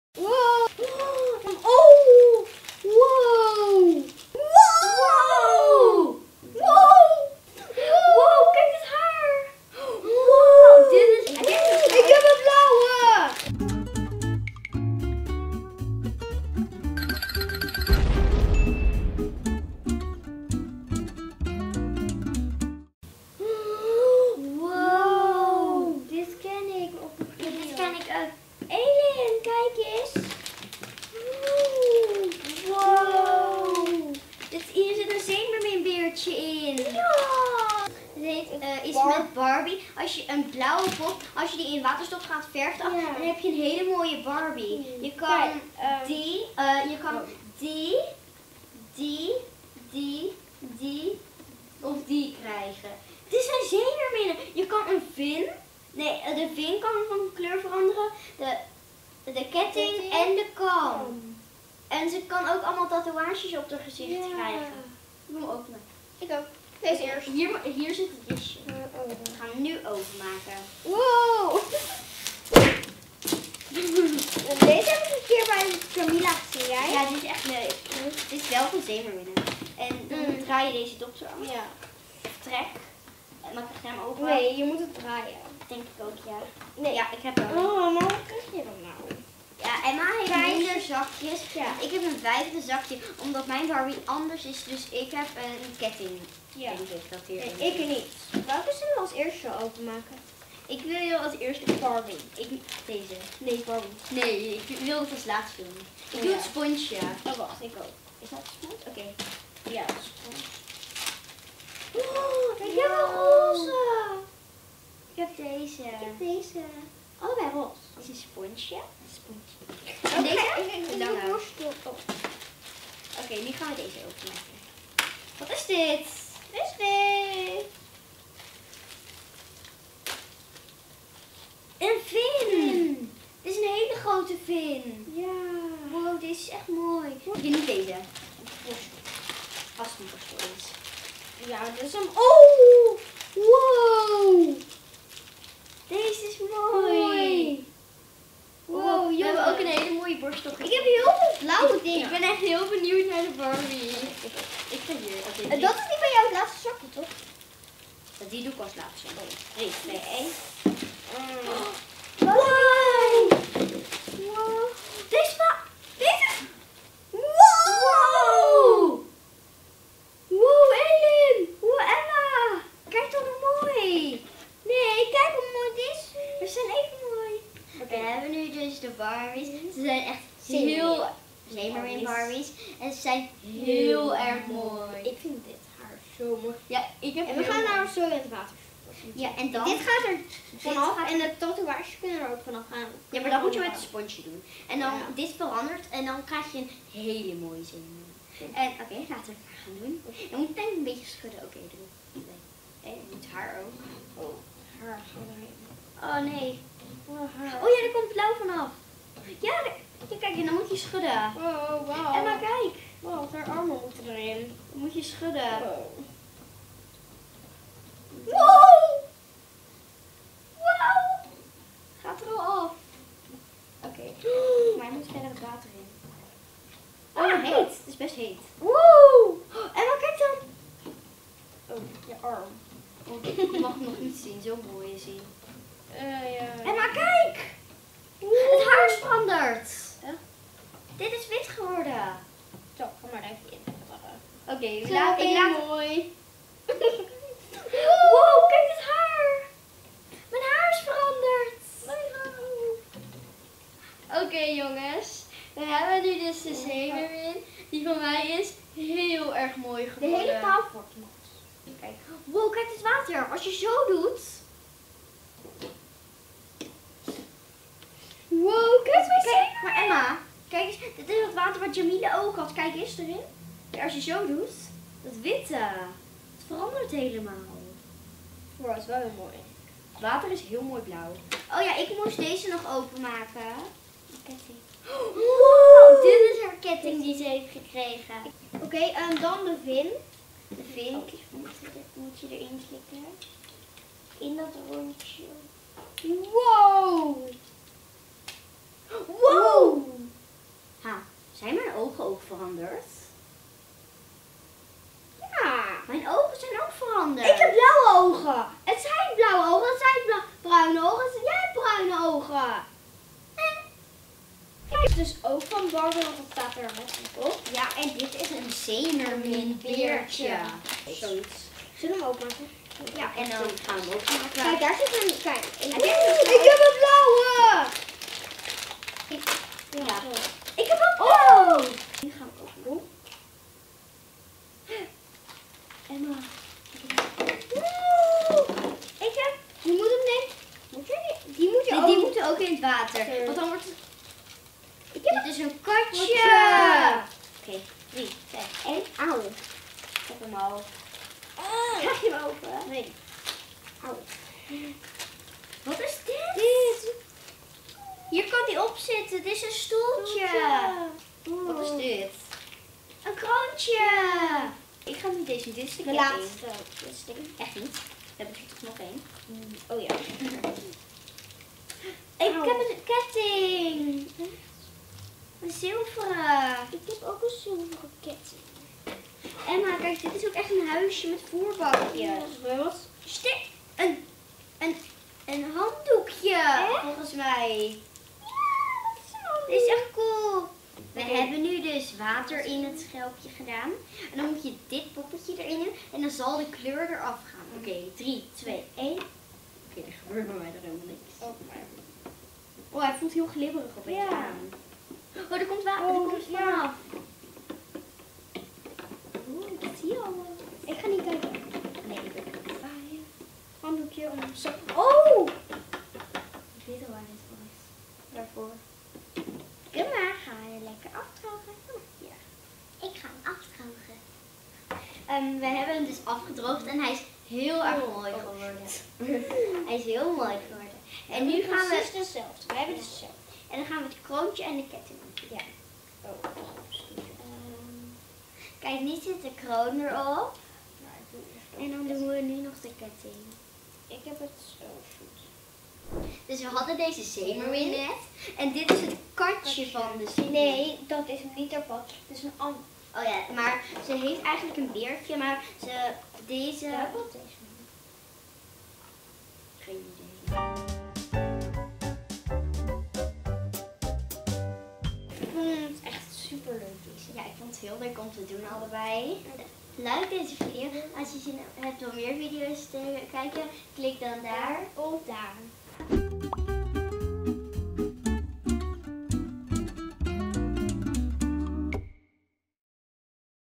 #179 Unboxing Barbie Color Reveal Wave 4 Zeemeermin en Dierenvriendjes - Proefmonsters
Proefmonster Ellin en Emma unboxing Barbie Color Reveal. Mega make-overs met alleen maar WATER! Welke Barbie Zeemeermin en Dierenvriendjes zullen zij onthullen?

*Elke dinsdagochtend om 10:00 een nieuwe aflevering! *